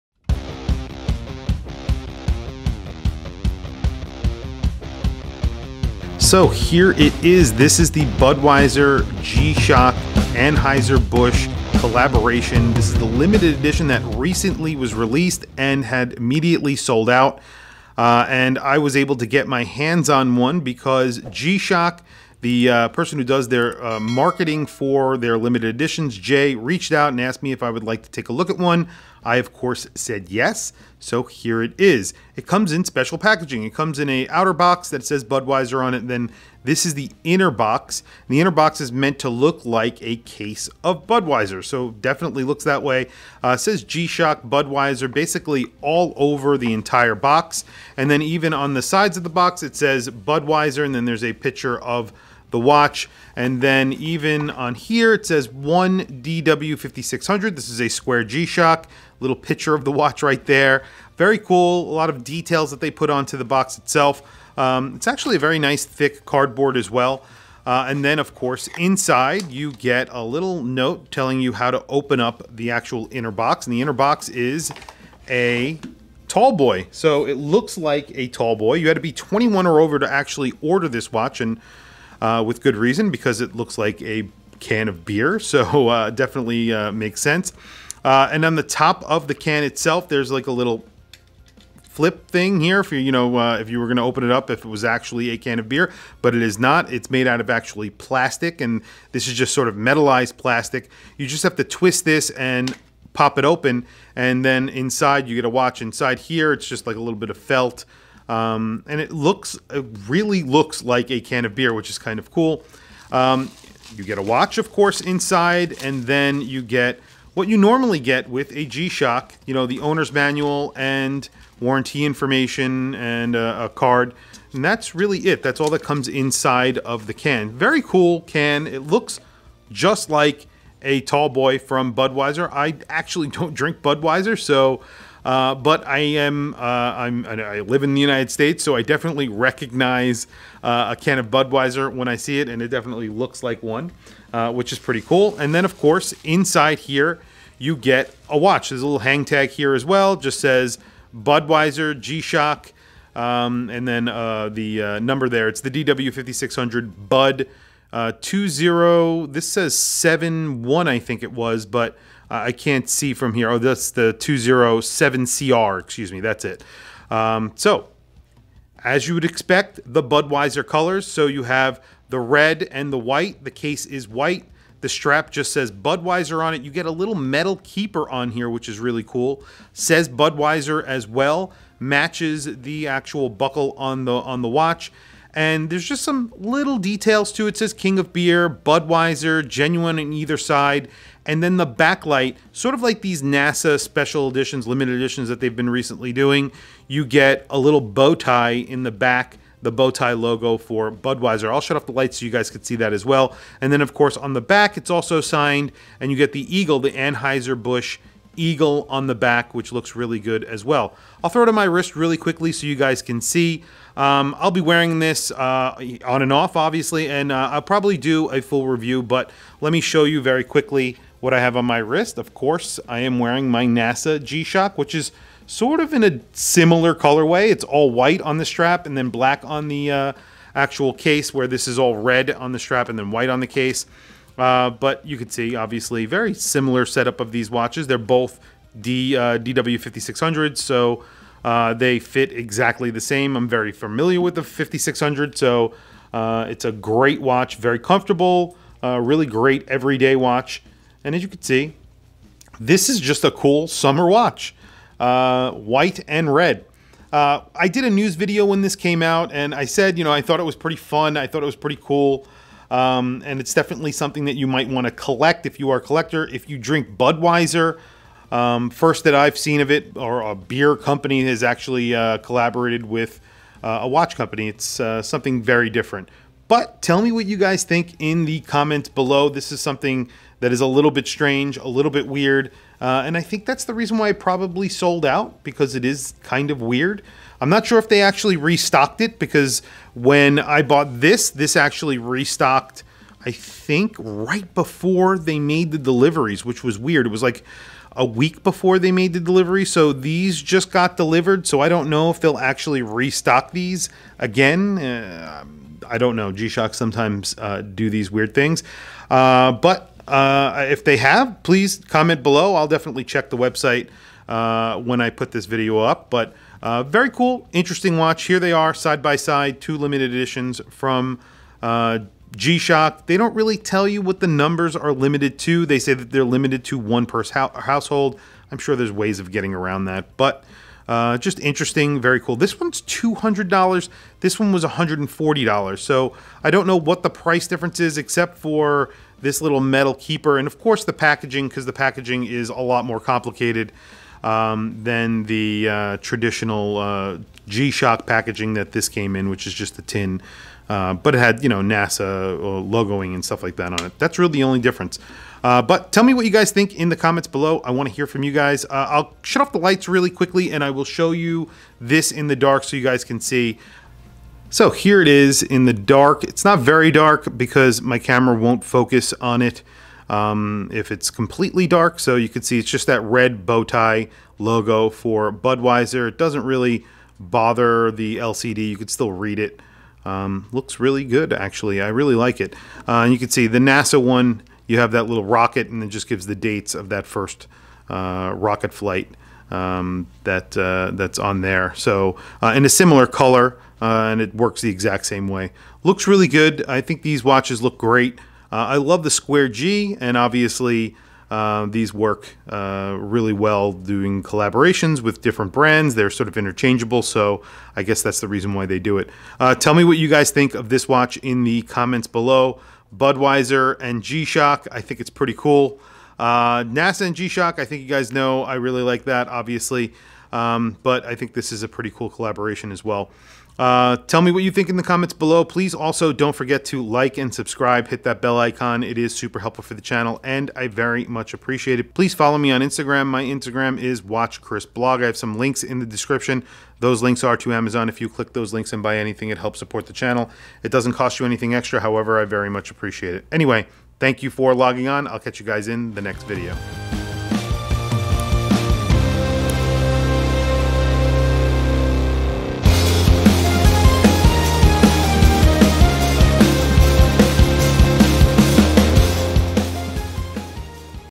Hands On Unboxing G SHOCK X BUDWEISER Limited Edition DW5600BUD20 DW5600 Square Bud G-shock Square
Also if you liked the shirt I was wearing in this video check out Into The AM using the below link!